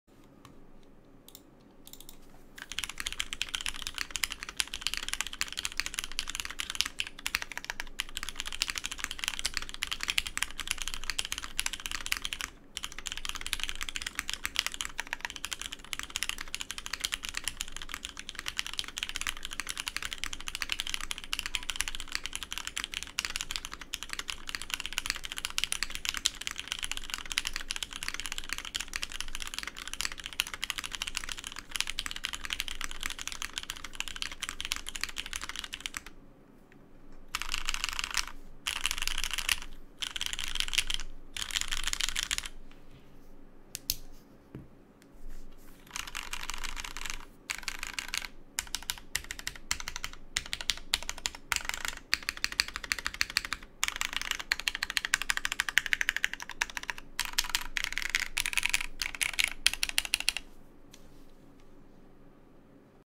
[Soundtest] Matrix 2.0 OG with lubed Cherry Hyperglide Blacks
Personal build

Keyboard: Matrix 2.0 OG in light pink
Plate: Aluminum
Switches: Hyperglide blacks [205g0/TX films/TX 60g Long]
Stabilizers: Zeal stabs  [205g0]
Keycaps: GMK WoB + WoB Hangul
Dampening: None